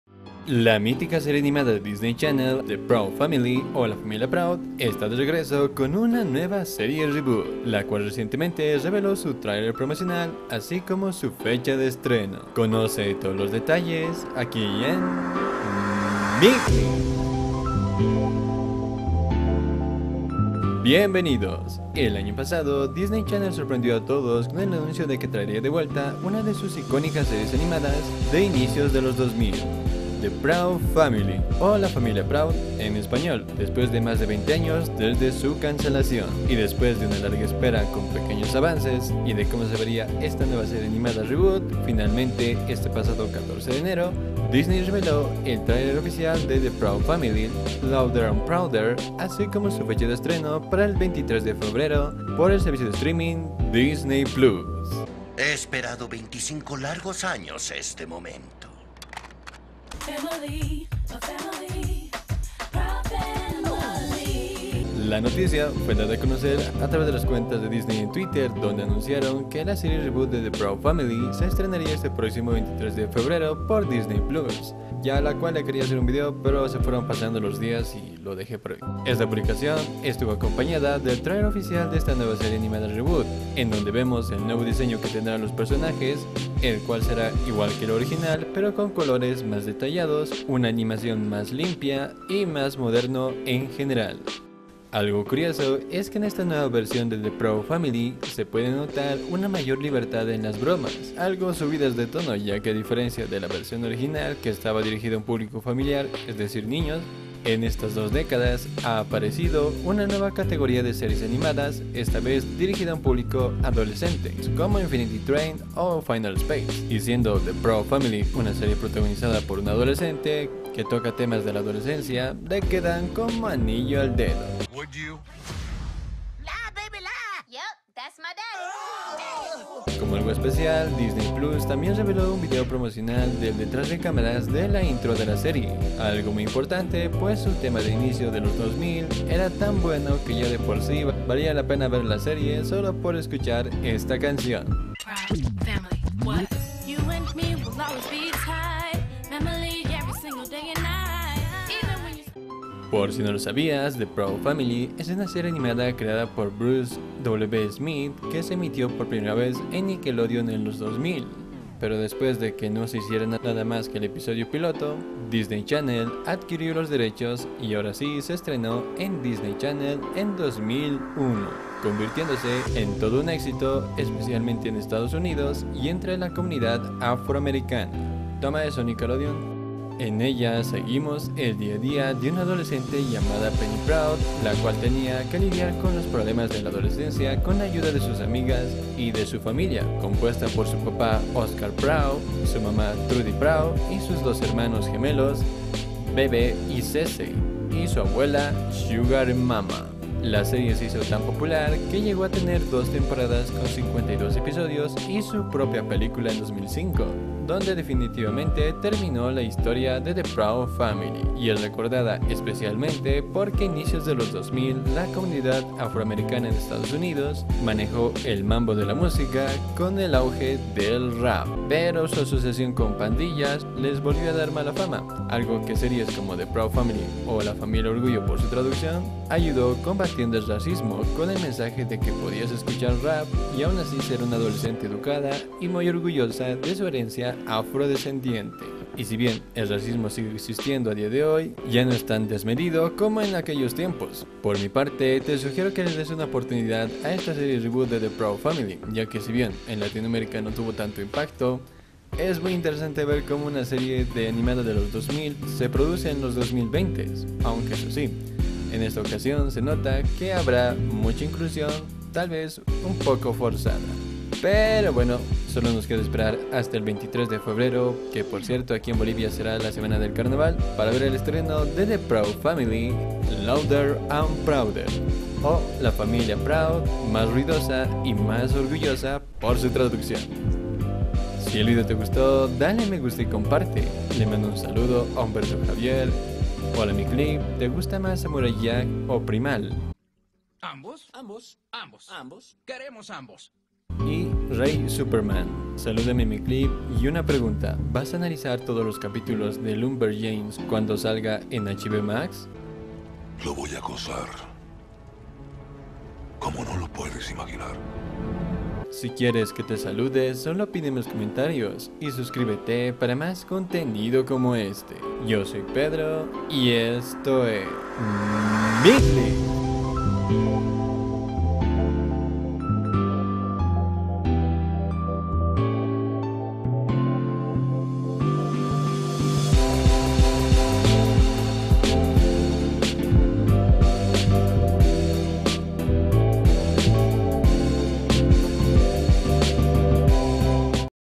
LA FAMILIA PROUD REGRESA con SERIE REBOOT | FECHA DE ESTRENO
Hola a todos la iconica serie animada de disney channel de incios de los 2000 the proud family, o la familia proud esta regresando con una serie aniamda reboot para disney plus, y en este video te dire su fecha de estreno y las curiosidades del trailer.